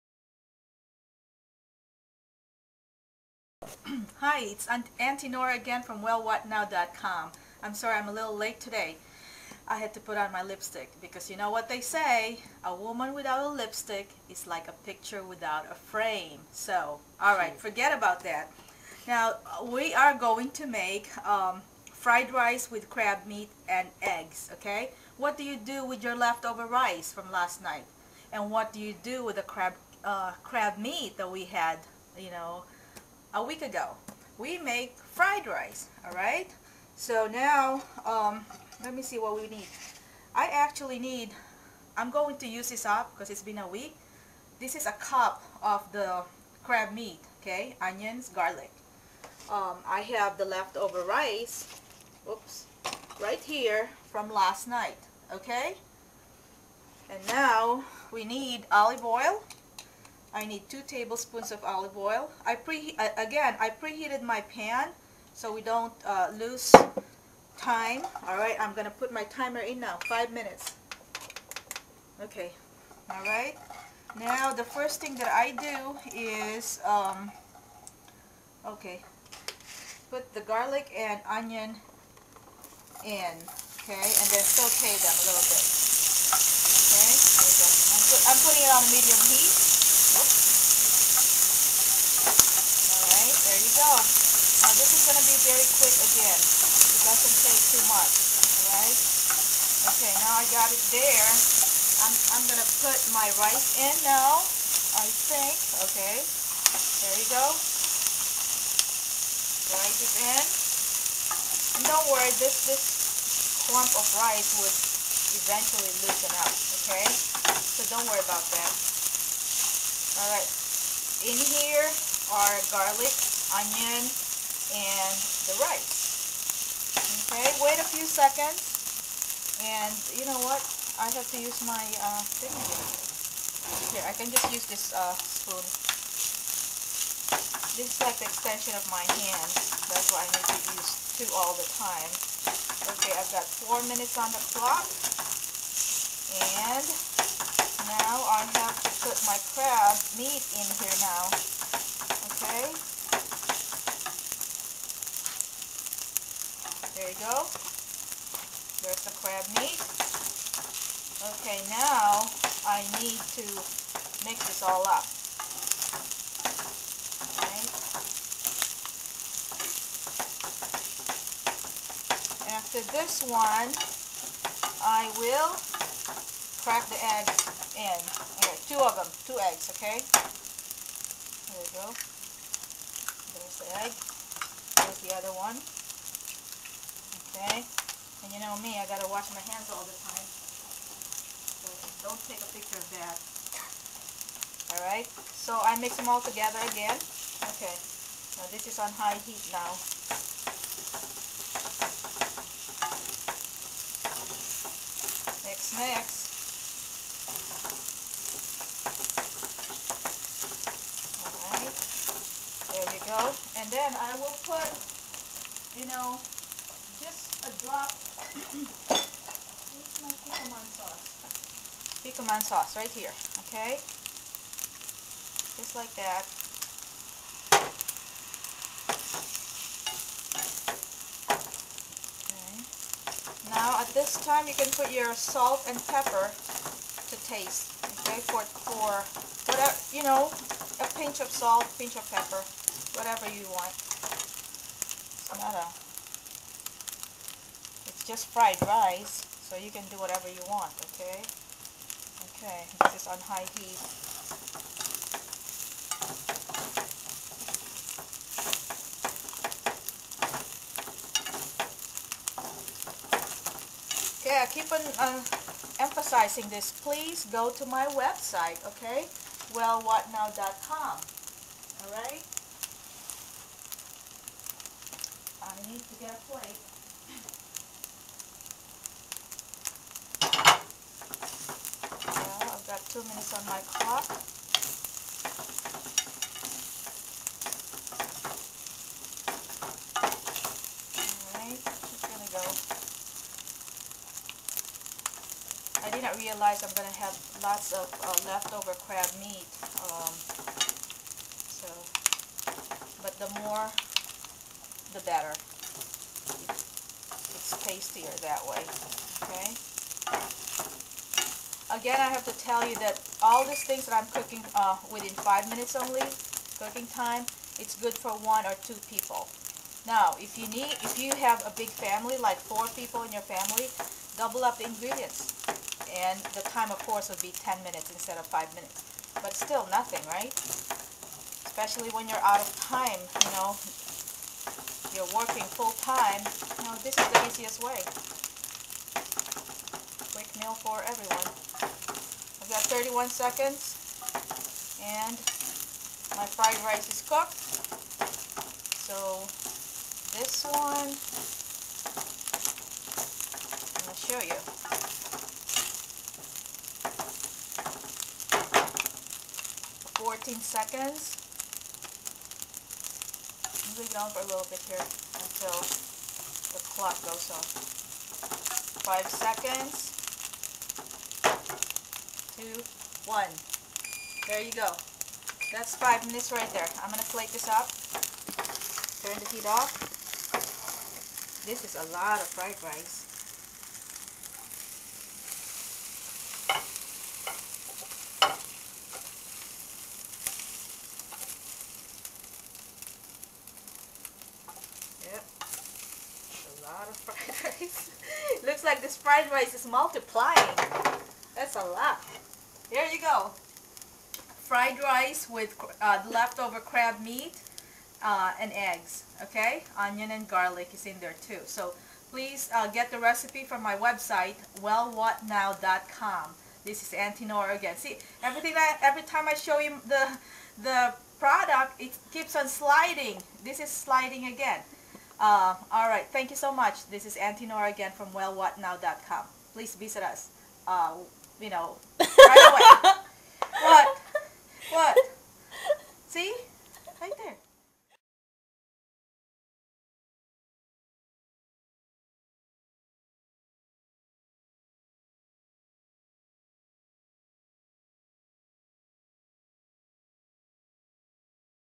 5 Minutes Cooking with Auntie Nora: Crab Fried Rice
-1 cup canned crab meat
-2 eggs
-3 cups leftover cooked rice
-2 garlic cloves
-1 small onion
-2 tbsps olive oil
-1 tbsp soy sauce

In a medium non-stick frying pan, pour in olive oil and put on medium heat.  Add in garlic and onion, then saute.  Next add in the rice and stir to mix.  Then add in crab meat, eggs, and seasonings.